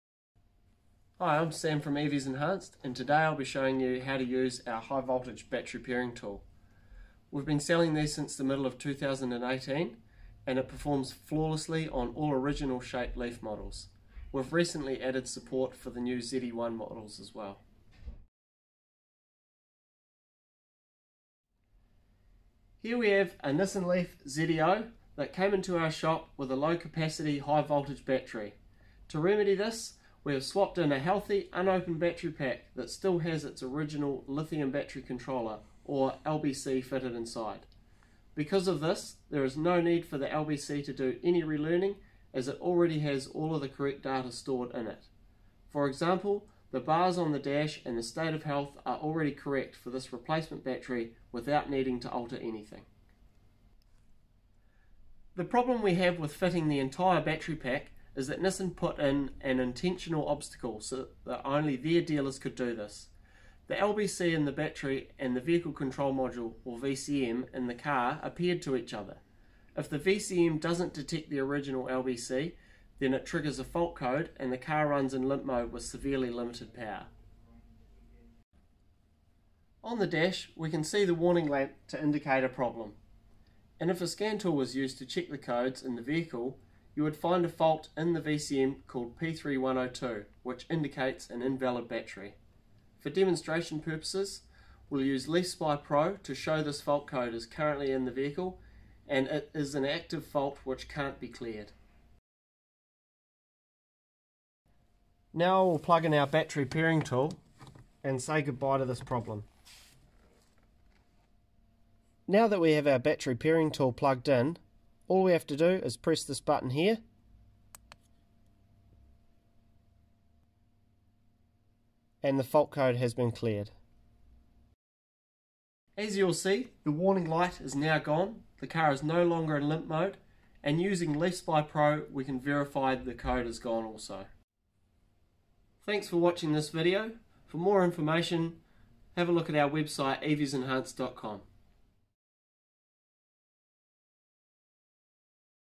Nissan Leaf HV Battery Pairing Tool
Demonstrates using our pairing tool to perform battery registration when a new or used replacement battery is fitted to a Nissan Leaf. This eliminates the fault code P3102 which relates to battery swaps.